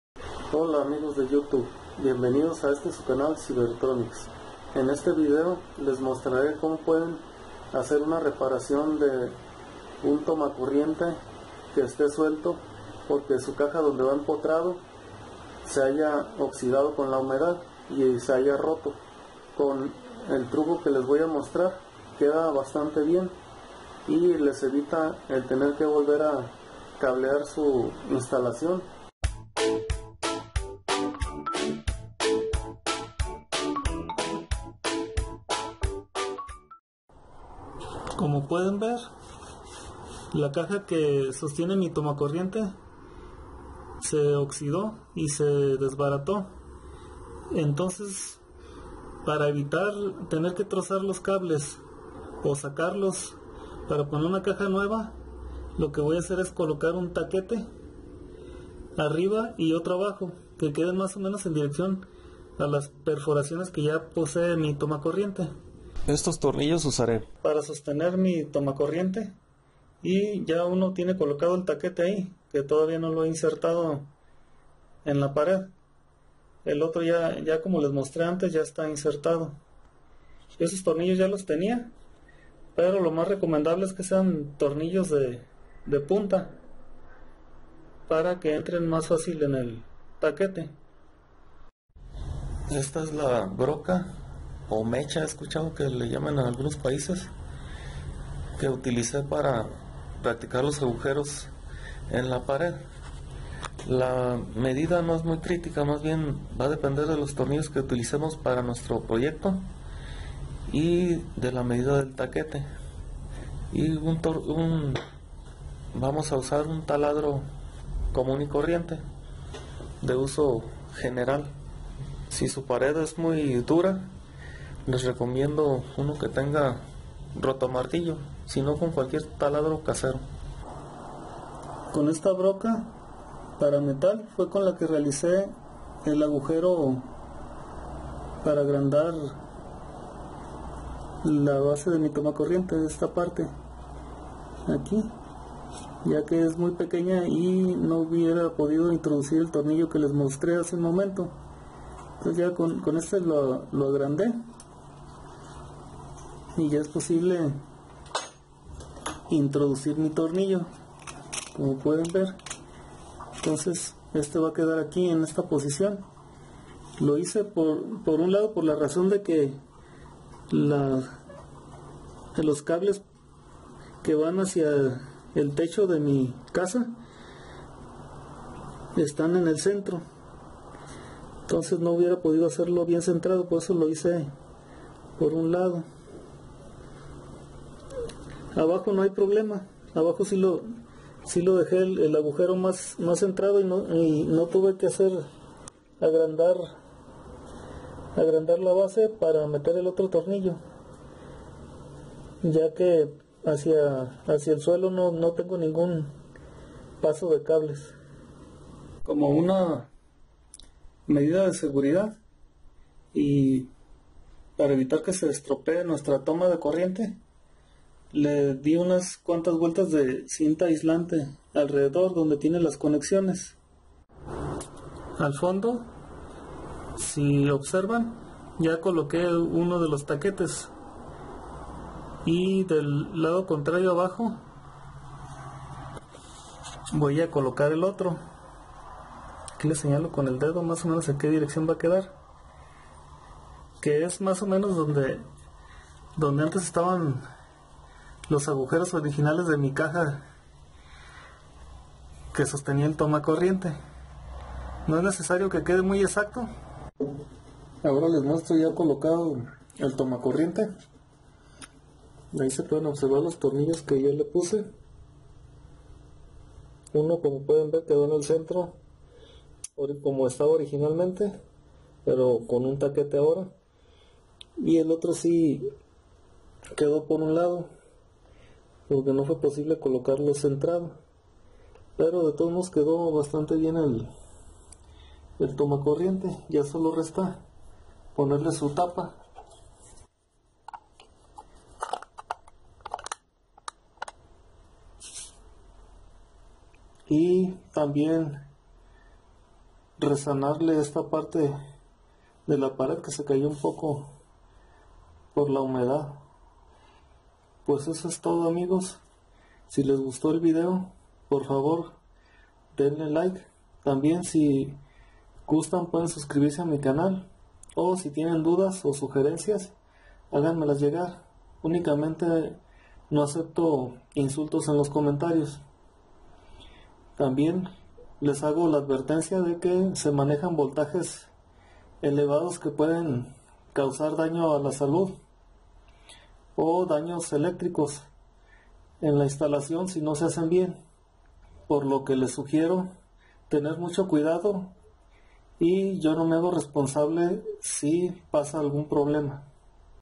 🔌⚡COLOCAR TOMACORRIENTE CON BASE OXIDADA Y ROTA
EN ESTE VÍDEO LES MUESTRO COMO ARREGLE UN TOMACORRIENTE QUE ESTABA SUELTO DEBIDO A QUE LA BASE (EN MÉXICO ALGUNOS LE DICEN CHALUPA) DONDE ESTABA EMPOTRADO SE OXIDO POR LA HUMEDAD. LO HICE SIN NECESIDAD DE CORTAR CABLES O SACARLOS PARA PONER OTRA CAJA.